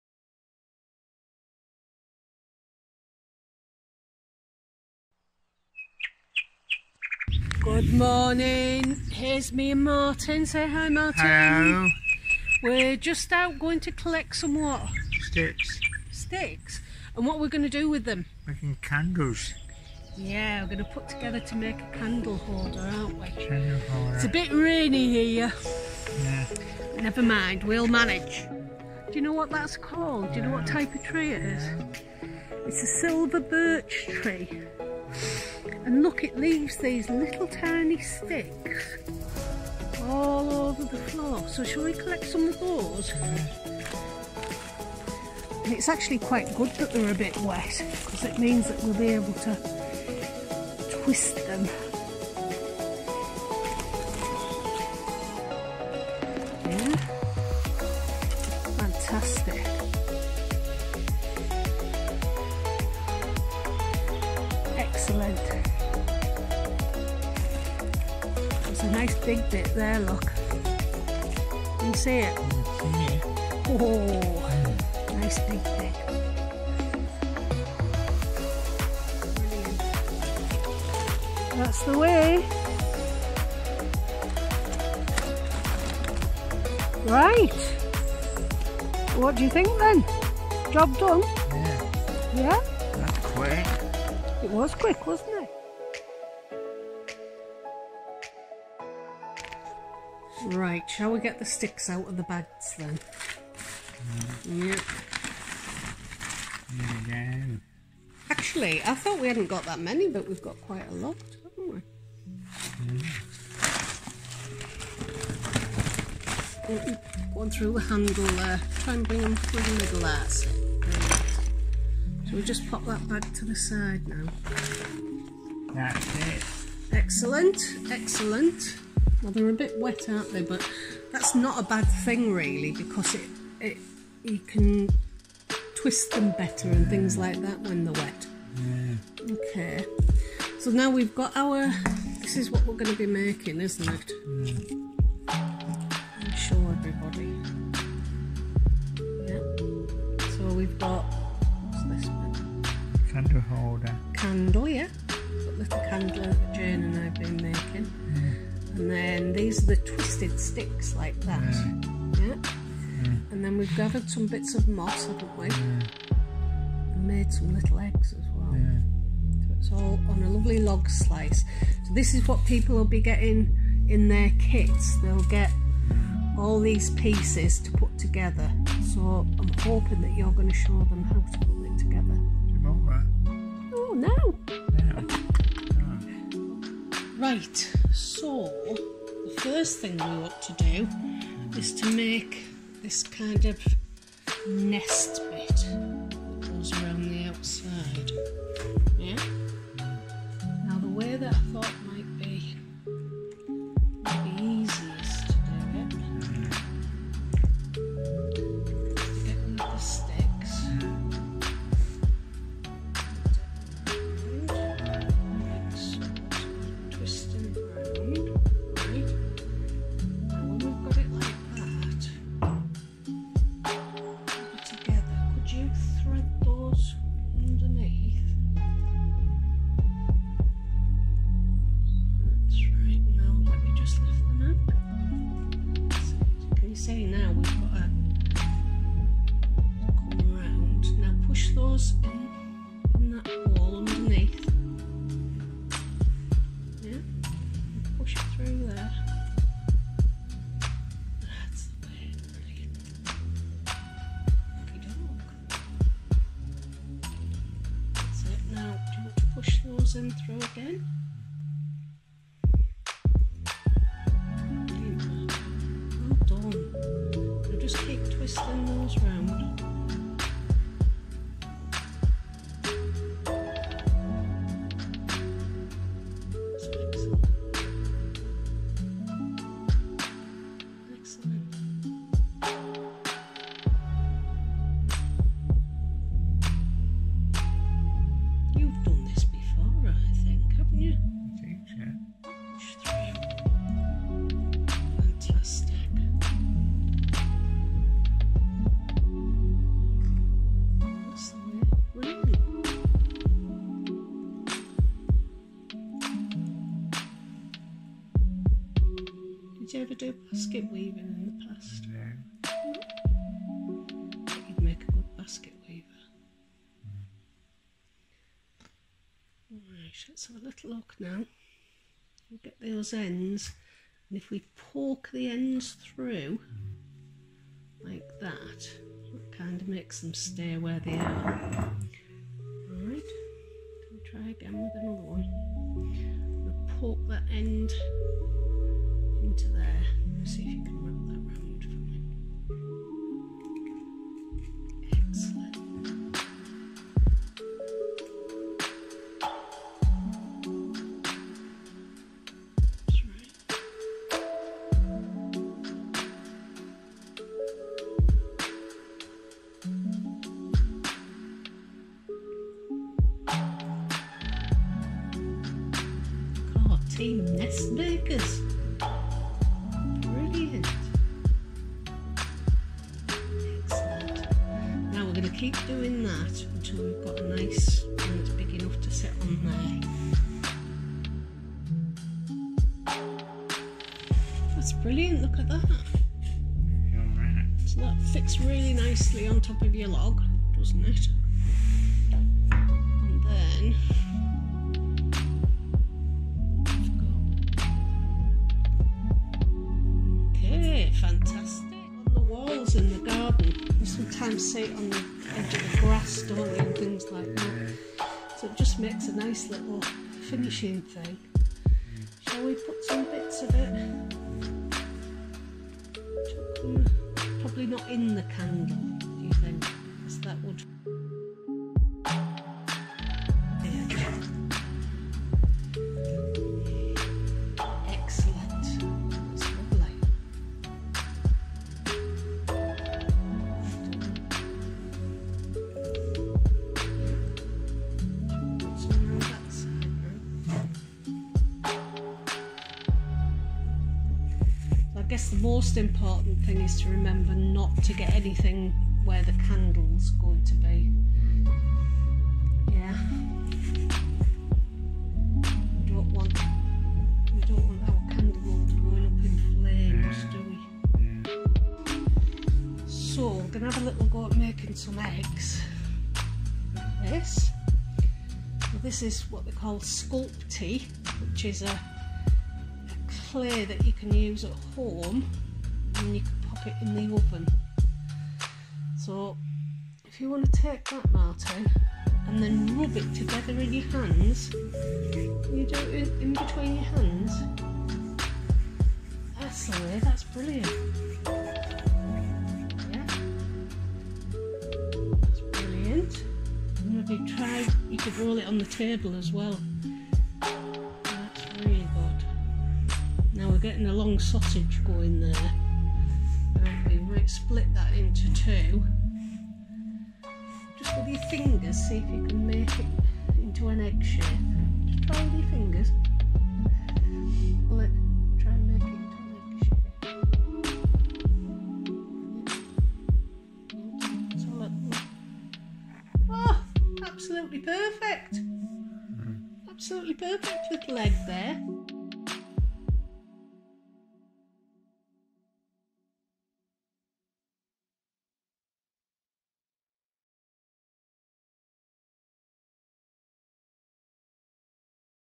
How to: Make an Easter candle holder
Join Sarah and Martin as they show you how to make a candle holder. The candle holder makes the perfect Easter table decoration and you can find everything you need at home!

You will need:

Some twigs, collect them from the garden or while out on a walk.
A small jam jar
A tealight candle
Blu Tack